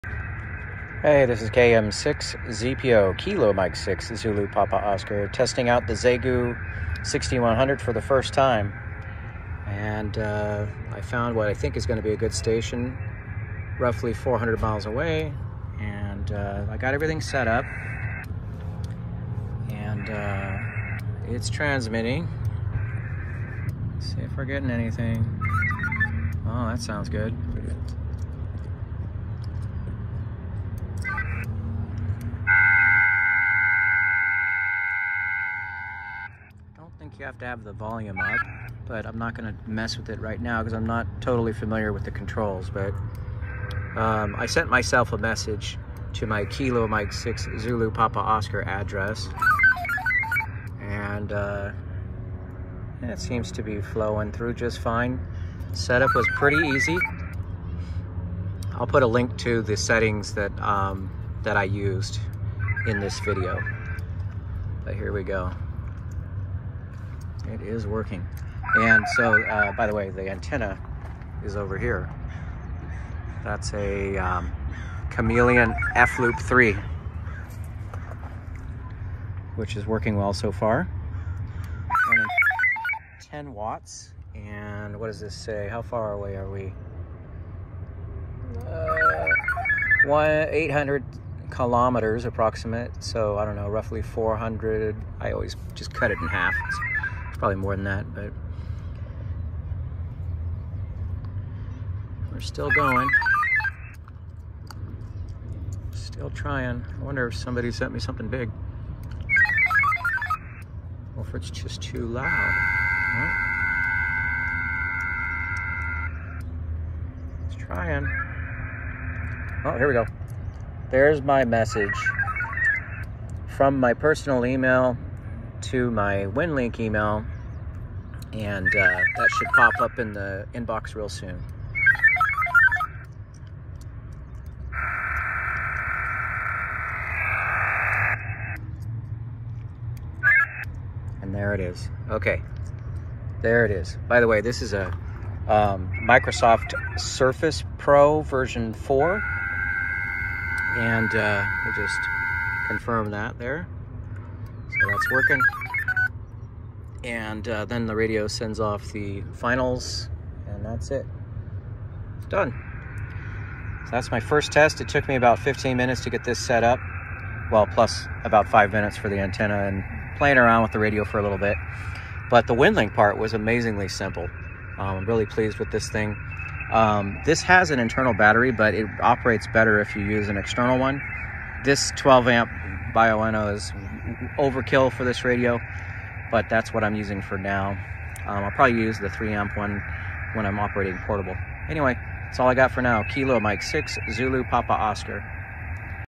Winlink VARA HF with Xiegu x6100
First attempt with a brand new Xiegu X6100 to do Winlink VARA over HF was easy.  The computer recognized the radio right away.  I used these settings